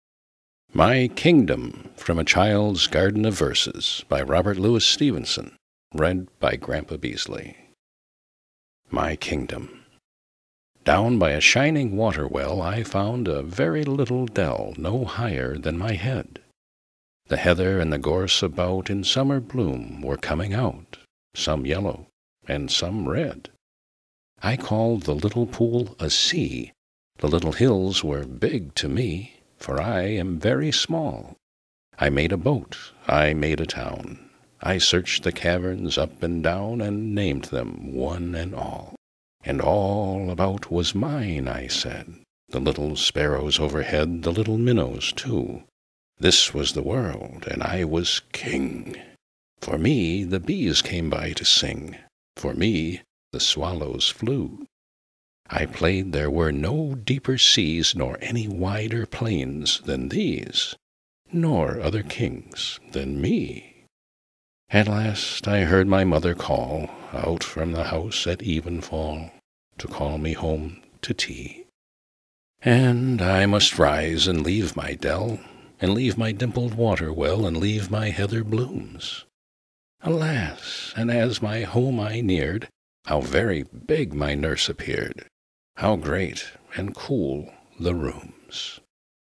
My Kingdom
My Kingdom from A Child's Garden of Verses by Robert Louis Stevenson read by Grampa Beesley